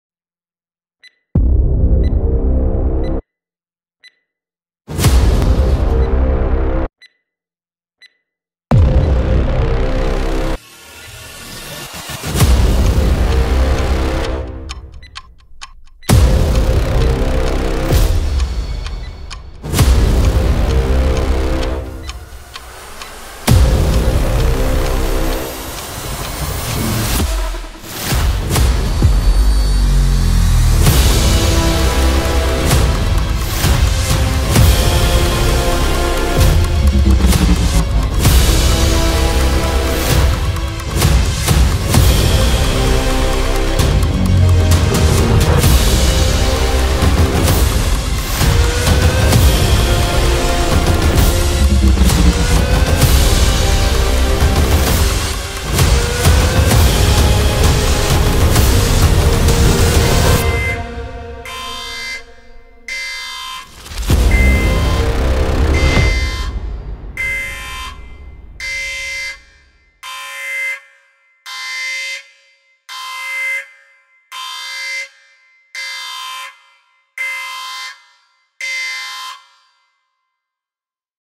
1 Minute Timer (EPIC VERSION)
A 1 minute timer that I made myself that is completely for you to use to time whatever you are timing. Have fun!